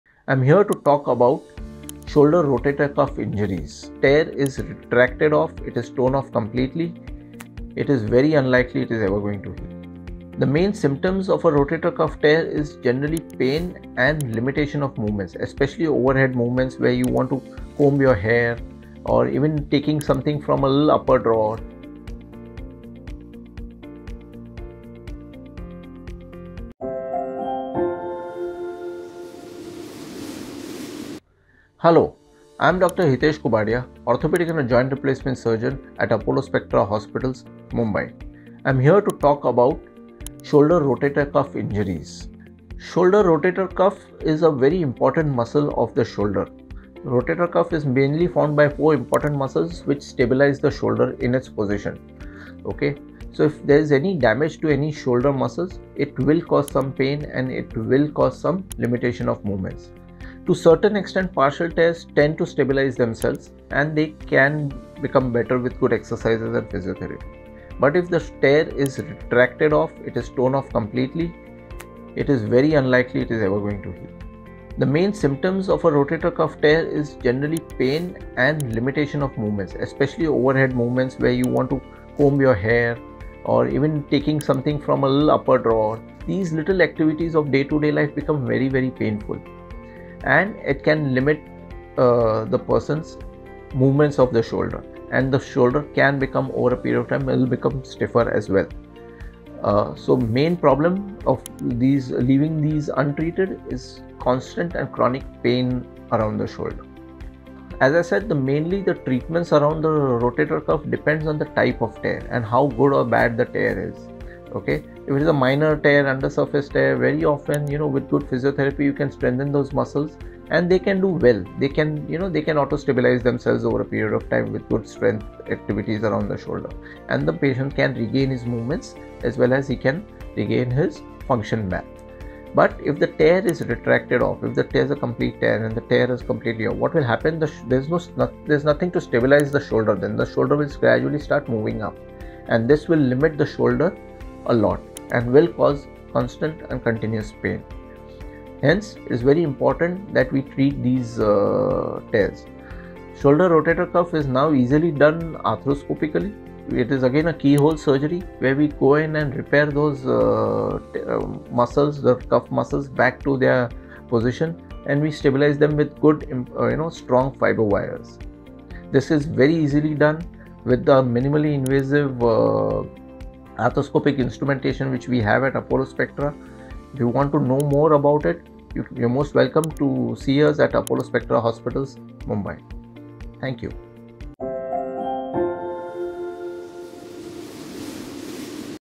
Shoulder Rotator Cuff Tears: Causes, Symptoms & Treatment
A partial or complete rotator cuff tear makes it difficult to raise and move your arm. Listen to Dr. Hitesh Kubadia as he explains about Shoulder Rotator Cuff, its functions along with the Tears caused to the Shoulder Rotator Cuff. The video also comprises of the symptoms, surgeries and Physiotherapy treatment required for the betterment of the Shoulder Rotator Cuff.

0:43- What is Shoulder Rotator cuff? What is its function?
1:13-  What are symptoms for Shoulder Rotator Cuff Tear?
1:33- what are the effects of Shoulder Rotator Cuff tear?
1:45- If a Shoulder Rotator Cuff tear is untreated, what its consequences?
1:51- What are the treatments available for Rotator Cuff tear?
1: 56- What is the treatment for Minor Shoulder Cuff Tear?
2:14- What is the treatment for Major Shoulder Cuff tear?
2:41-  What is Minimally Invasive Arthroscopy Surgery for Shoulder Cuff Tear?